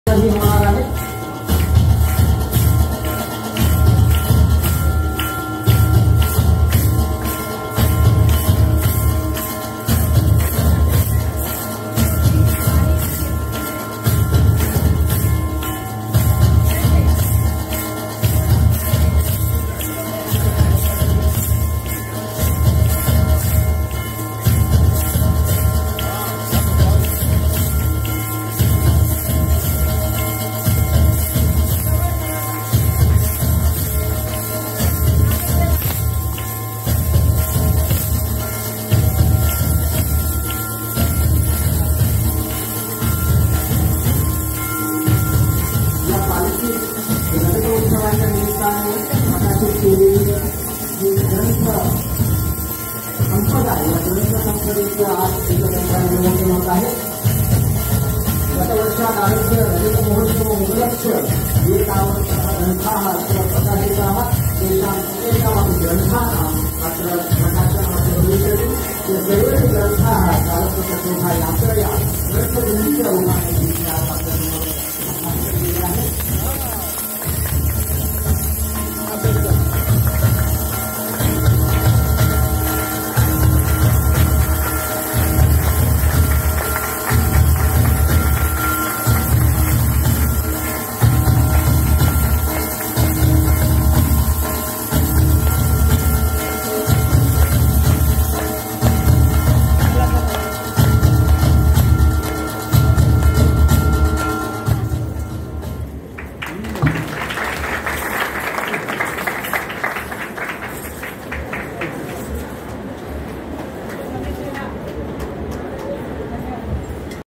Book Release Ceremony At Kavikulaguru Kalidasa Sanskrit University, Ramtek
Book Release Ceremony At
Kavikulaguru Kalidasa Sanskrit University, Ramtek, Nagpur, Maharashtra
on 18/09/2022